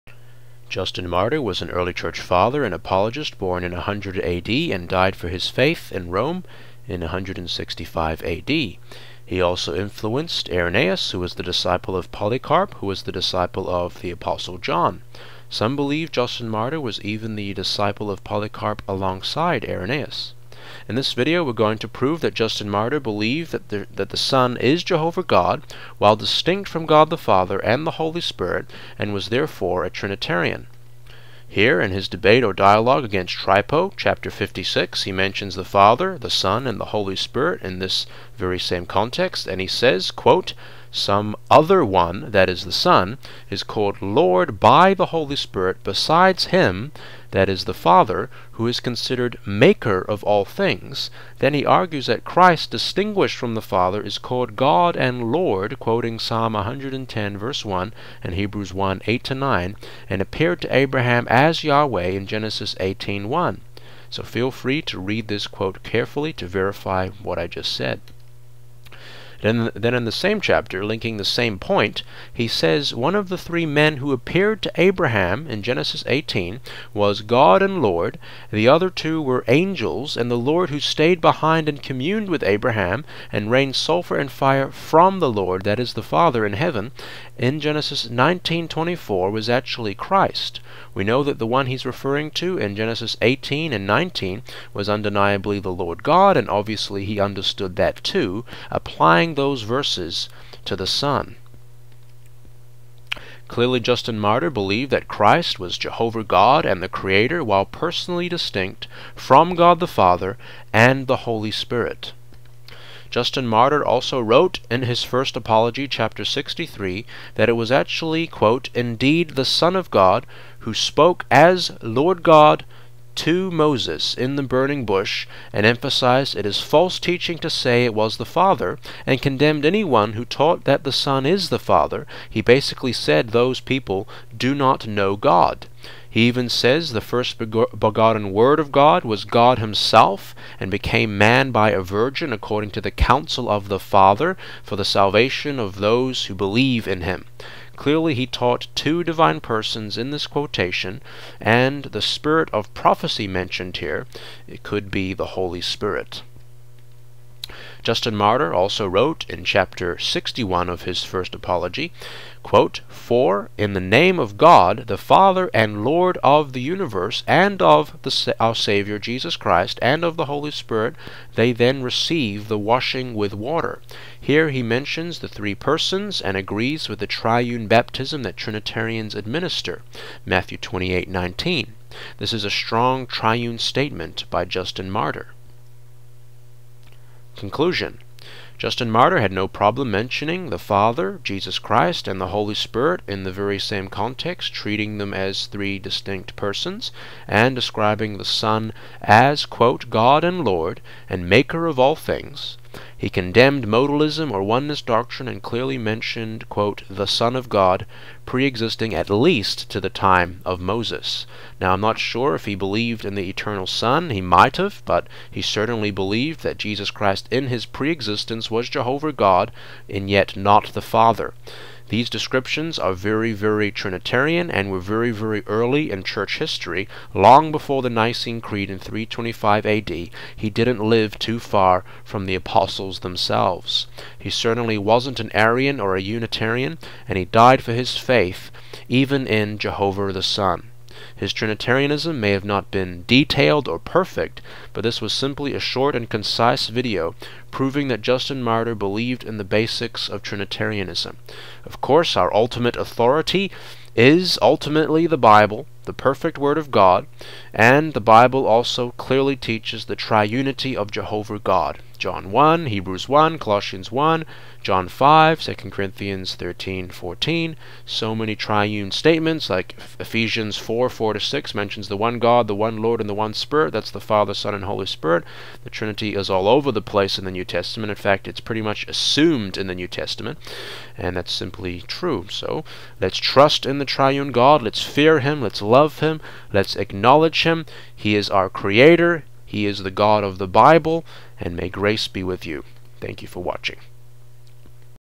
Justin Martyr (c. 100-165 AD) was a Trinitarian
Let's prove early church father, Justin Martyr (c. 100-165 AD) was a Trinitarian from his writings.